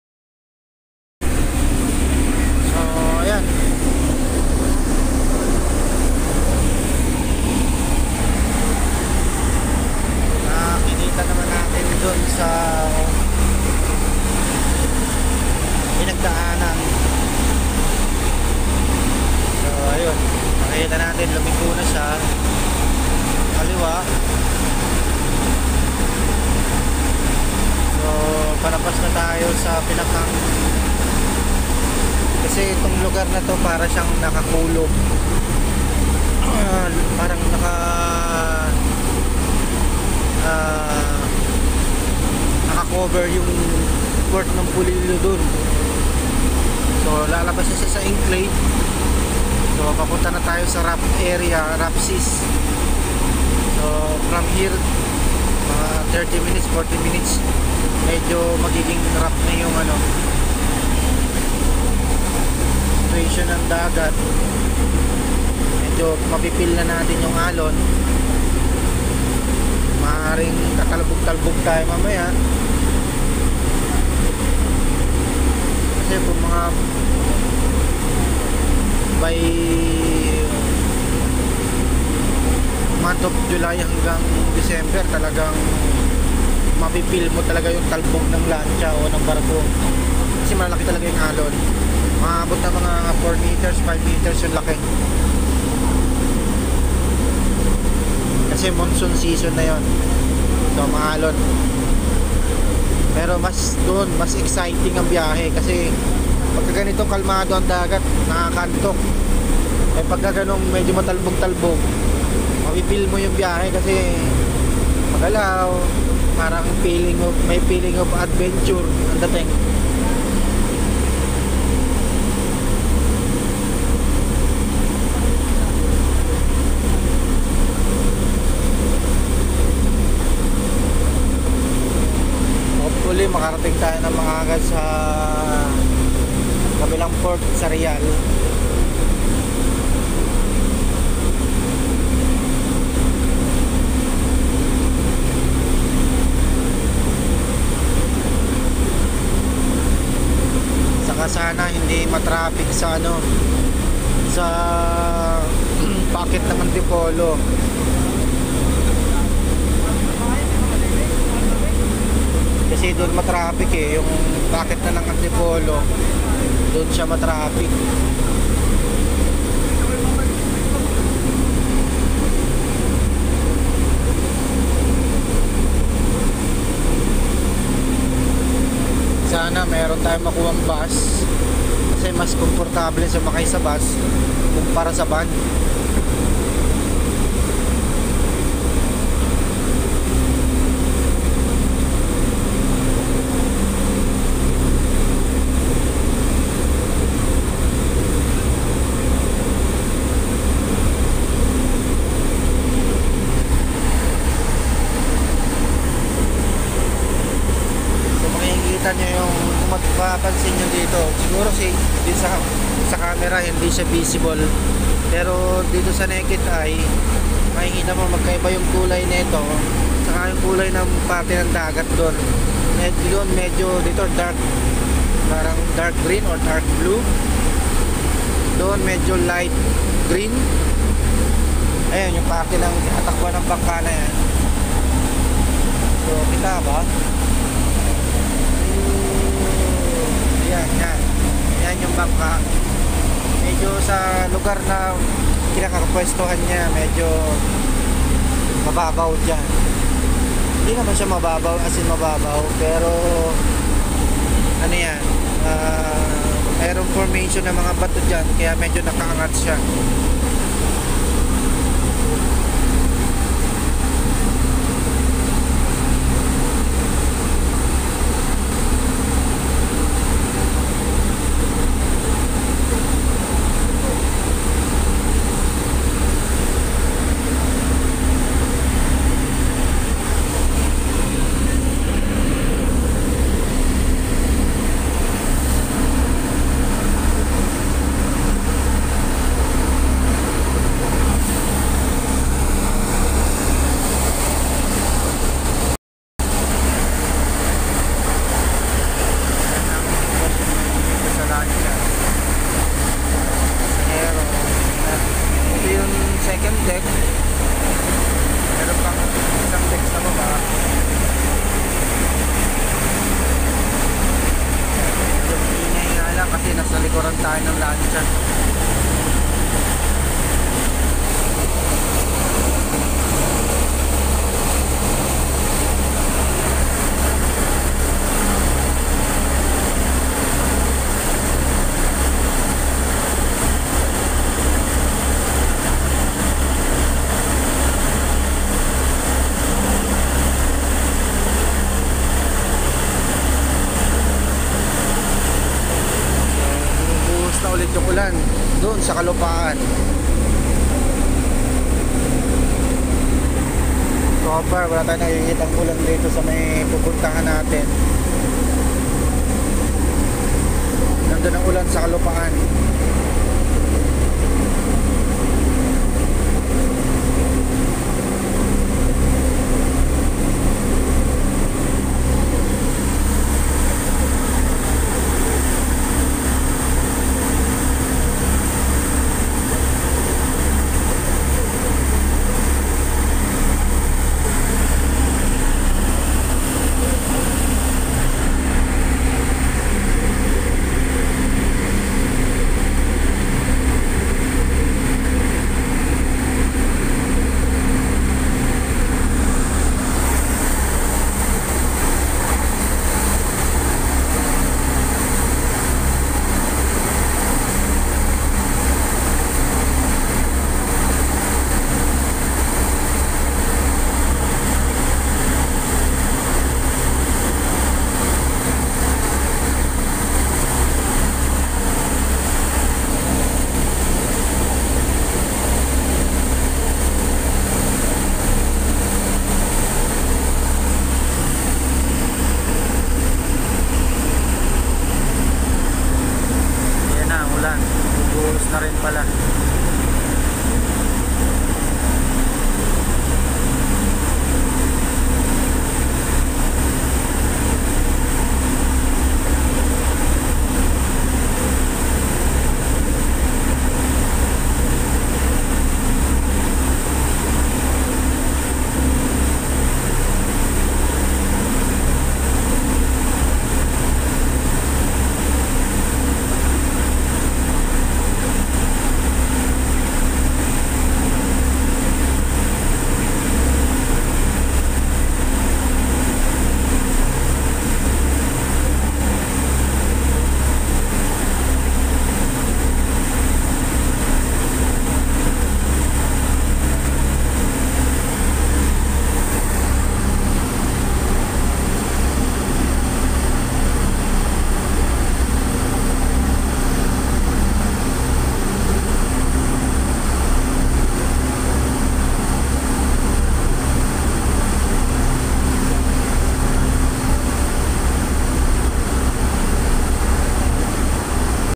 On board m/v Calucin going back to manila
Experiencing the moment in one of the oldest ship in Quezon#ship